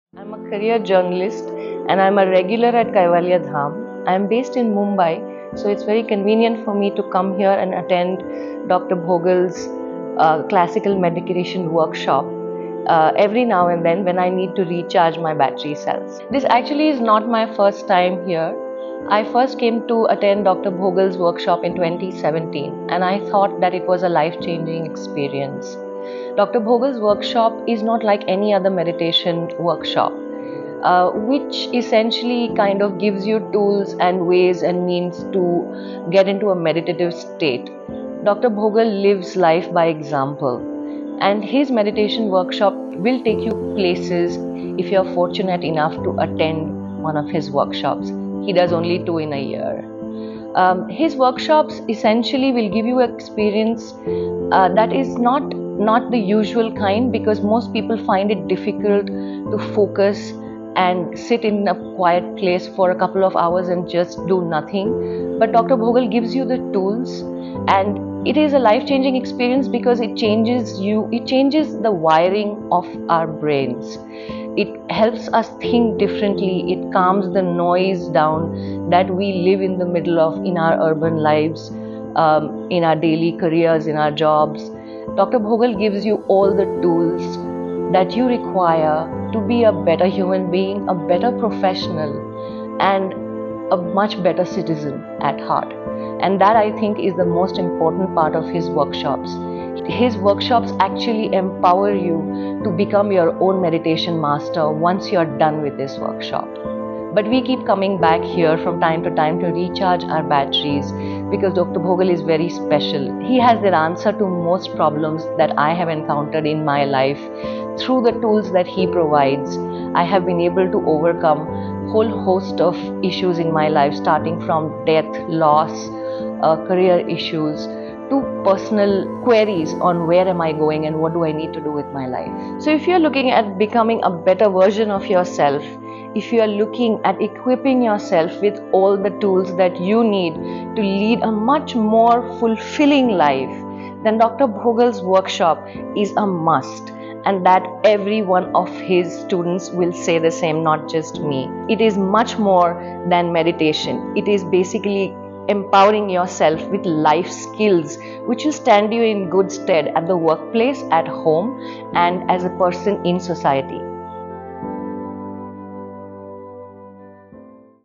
Classical Meditation for Holistic Health & Wellness | Dr. R. S. Bhogal Sir | April 2024
Ms. Malini Bhupta, a journalist from Mumbai, attended the Onsite Workshop - Classical Meditation for Holistic Health and Wellness for the fifth time.  She shared her experience in amazing words.